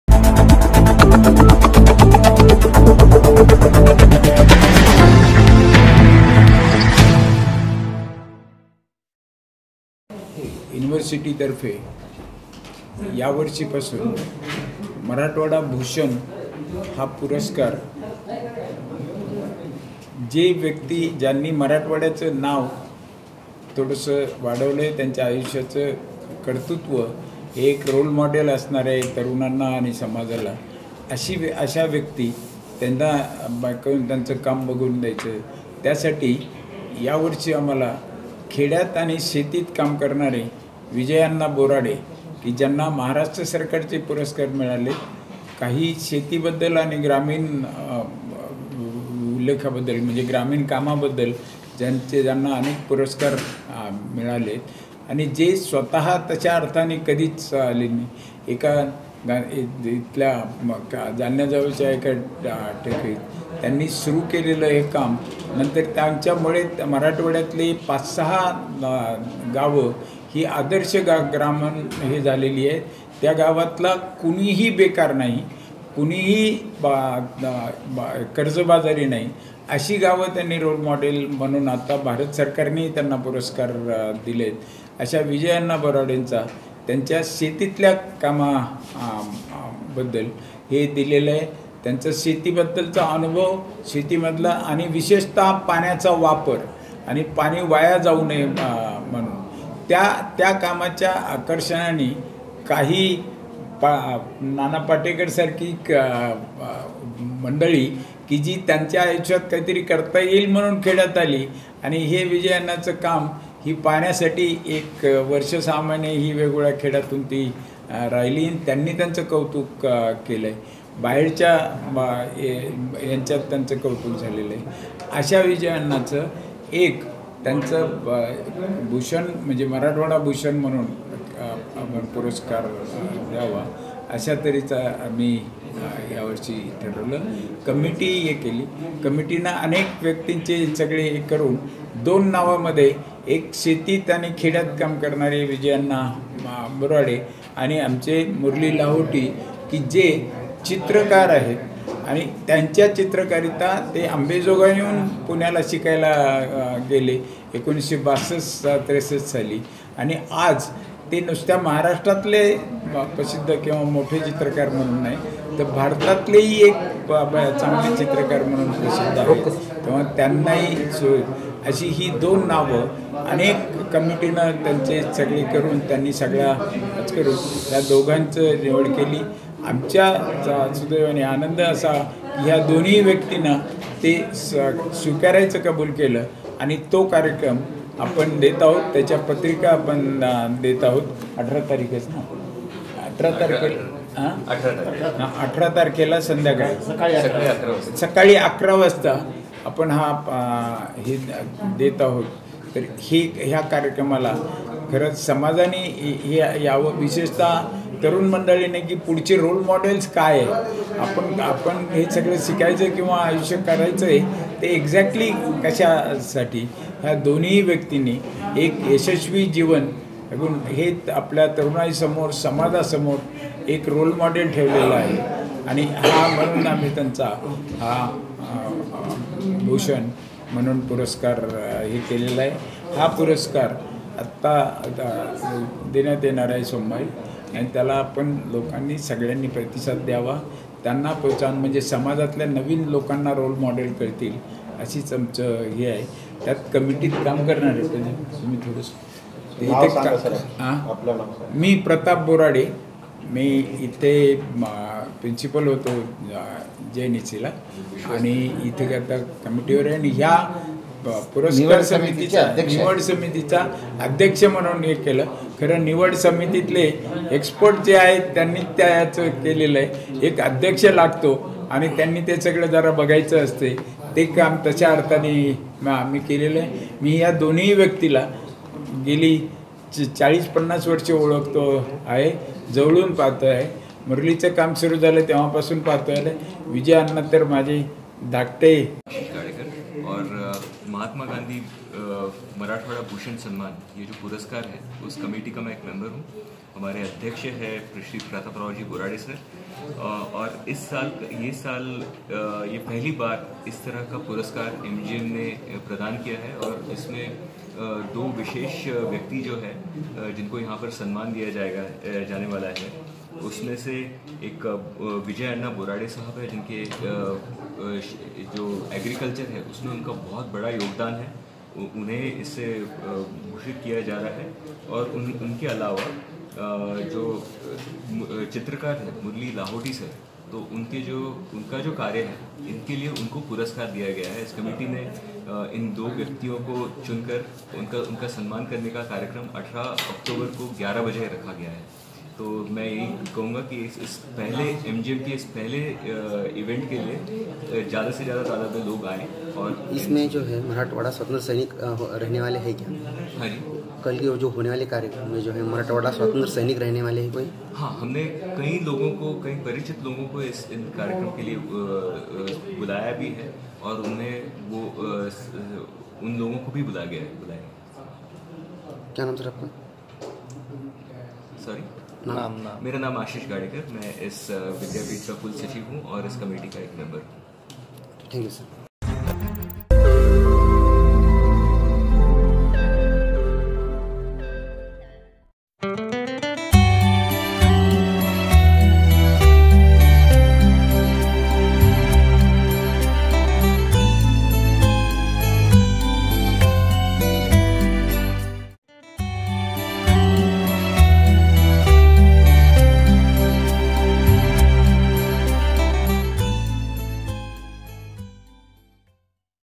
मराठवाड़ा भूषण की घोषणा 18 अक्टुबर को MGM में कार्यक्रम विजय बोराडे चित्रकार मुरली लाहोटी का सम्मान
मराठवाड़ा  भूषण की घोषणा 18 अक्टुबर को MGM में कार्यक्रम विजय अण्णा बोराडे चित्रकार मुरली लाहोटी का सम्मान